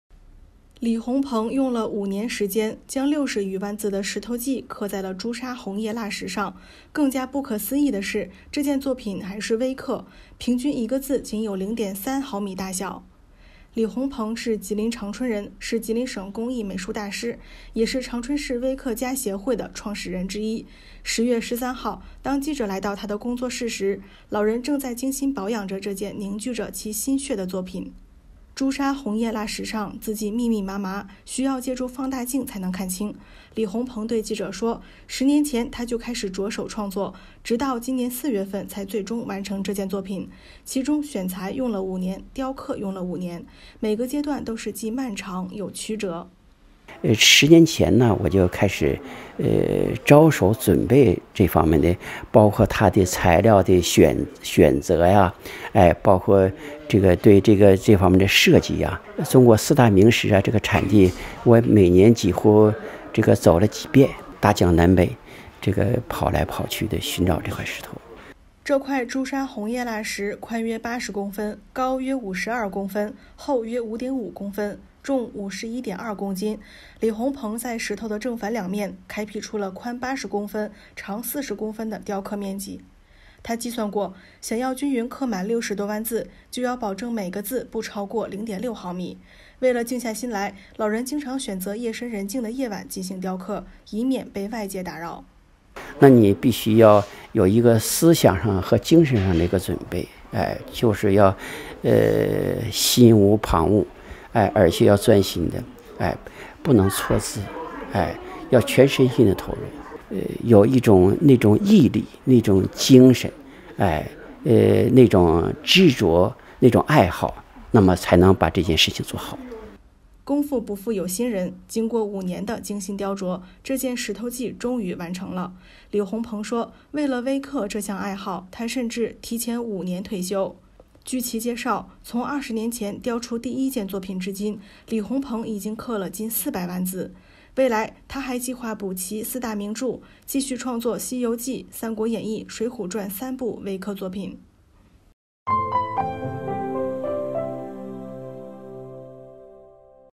字均仅0.3毫米！吉林老人5年微刻逾60万字《石头记》
李鸿鹏用了5年时间，将60余万字的《石头记》刻在了朱砂红叶蜡石上。更加不可思议的是，这件作品还是微刻，平均一个字仅有0.3毫米大小！